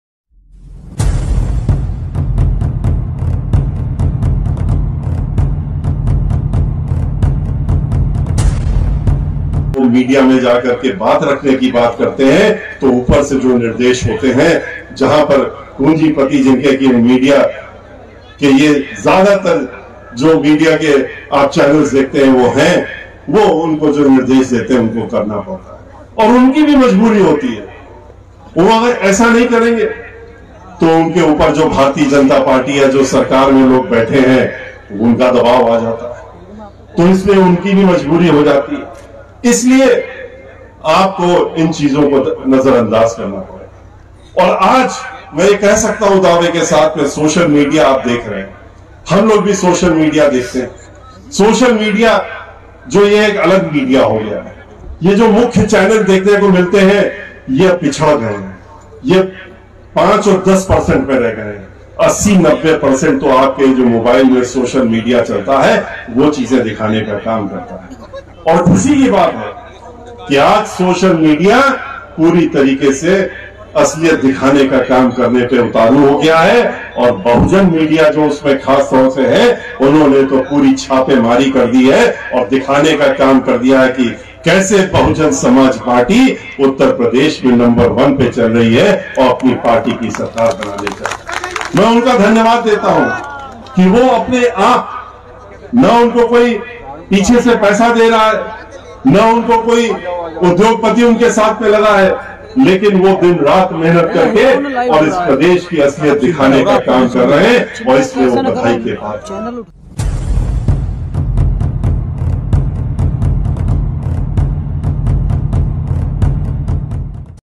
सतीशचंद्र मिश्रा ने की बहुजन मीडिया की तारीफ़,मनुवादी मीडिया की बैंड बजाई
About Video👇👇

{ In Thise Video Many News About Bahujan Community people . Bharat Dastak Giving Everyday New Video About Bahujan Community News . }